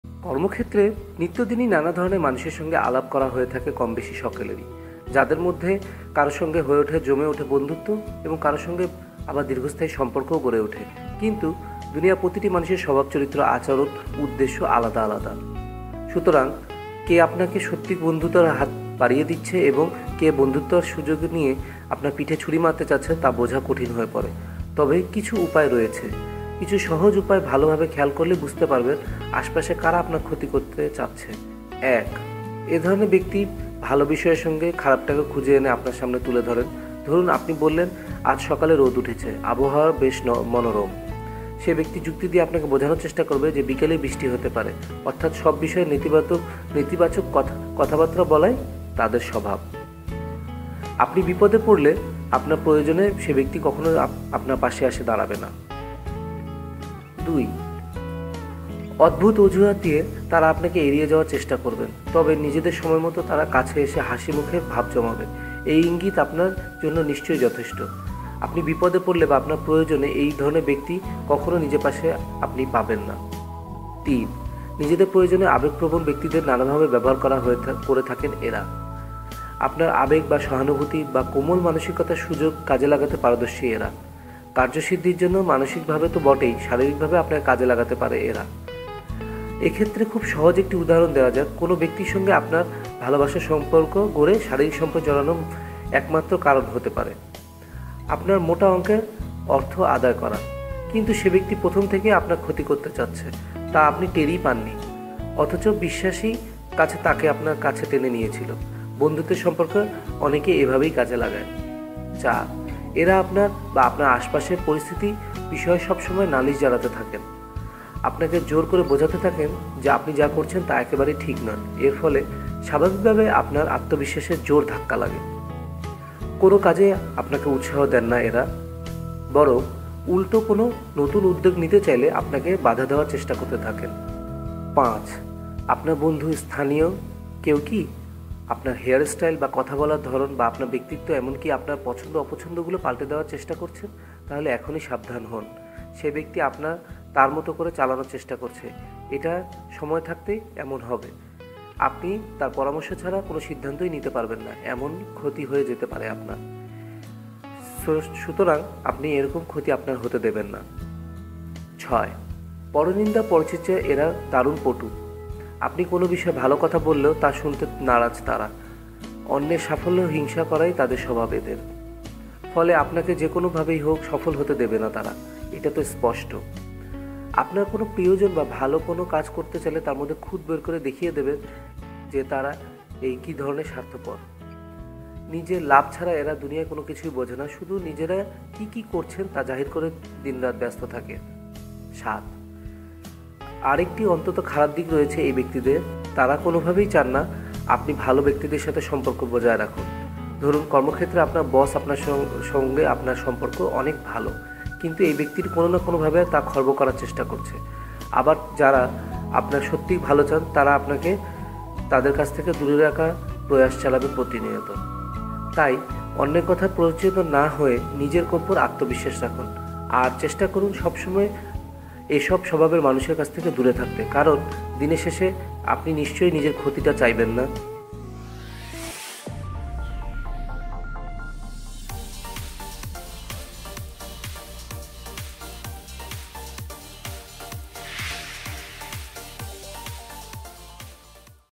কিভাবে বুঝবেন কারা আপনার ক্ষতি করতে চাইছে?
কোন ধরনের মানুষ আপনার ক্ষতি করতে পারে?

১. এ ধরনের ব্যক্তি ভালো বিষয়ের মধ্যে থেকেও খারাপটাকে খুঁজে এনে আপনার সামনে তুলে ধরেন। ধরুন, আপনি বললেন, আজ সকালে রোদ উঠেছে, আবহাওয়াটা বেশ মনোরম। সেই ব্যক্তি যুক্তি দিয়ে আপনাকে বোঝানোর চেষ্টা করবে বিকেলে বৃষ্টি হতেই পারে। অর্থাৎ সব বিষয়ে নেতিবাচক কথাবার্তা বলাই এঁদের স্বভাব।

২. আপনি বিপদে পড়লে বা আপনার প্রয়োজনে এই ধরনের ব্যক্তিদের কখনো নিজের পাশে পাবেন না।

অদ্ভুত অজুহাত দিয়ে তাঁরা আপনাকে এড়িয়ে যাওয়ার চেষ্টা করবেন। তবে নিজেদের সময়মতো তাঁরা কাছে এসে হাসিমুখে ভাব জমাবে। এই ইঙ্গিত আপনার জন্য নিশ্চয়ই যথেষ্ট!
৩. নিজেদের প্রয়োজনে আবেগপ্রবণ ব্যক্তিদের নানাভাবে ব্যবহার করে থাকেন এঁরা। আপনার আবেগ বা সহানুভূতি বা কোমল মানসিকতার সুযোগ কাজে লাগাতে পারদর্শী তাঁরা। কার্যসিদ্ধির জন্য মানসিকভাবে তো বটেই, শারীরিকভাবেও আপনাকে কাজে লাগাতে পারে এঁরা। এক্ষেত্রে খুব সহজ একটি উদাহরণ দেওয়া যাক। কোনো ব্যক্তির আপনার সঙ্গে ভালোবাসার সম্পর্ক গড়ে শারীরিক সম্পর্কে জড়ানোর একমাত্র কারণ হতে পারে, আপনার থেকে মোটা অঙ্কের অর্থ আদায় করা। কিন্তু সেই ব্যক্তি যে প্রথম থেকেই আপনার ক্ষতি করতে চাইছে, তা আপনি টেরও পাননি। সহজ বিশ্বাসেই তাঁকে কাছে টেনে নিয়েছিলেন। বন্ধুত্বের সম্পর্কেও অনেকে এভাবেই কাজে লাগায়।

৪. এঁরা আপনার বা আপনার আশেপাশের পরিস্থিতির বিষয়ে সবসময় নালিশ জানাতে থাকেন। আপনাকে জোর করে বোঝাতে থাকেন, আপনি যা করছেন তা একেবারেই ঠিক নয়। এর ফলে স্বাভাবিকভাবেই আপনার আত্মবিশ্বাসে জোর ধাক্কা লাগে। কোনো কাজেই আপনাকে উৎসাহ দেন না এঁরা। বরং উল্টো কোনো নতুন উদ্যোগ নিতে চাইলে আপনাকে বাধা দেওয়ার চেষ্টা করতে থাকেন